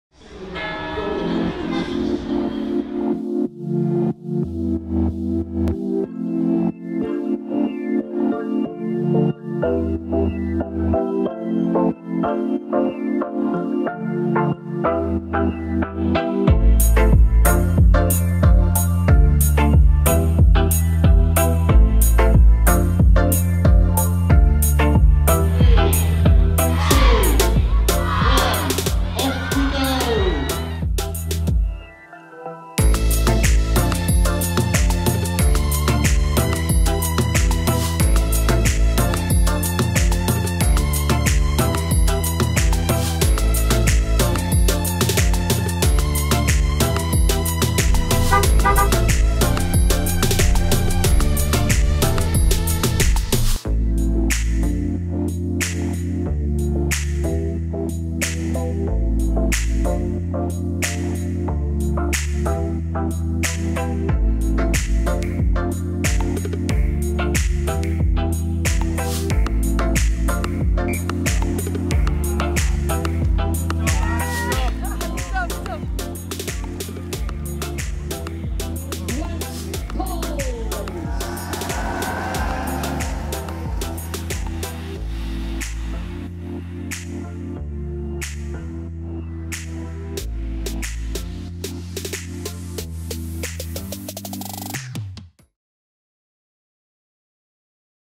Flavour of the Day | Tour of Britain 2018 Stage Six
Check out a flavour of Stage Six, our second Cumbrian stage of the 2018 Tour of Britain, from Barrow in Furness to Whinlatter.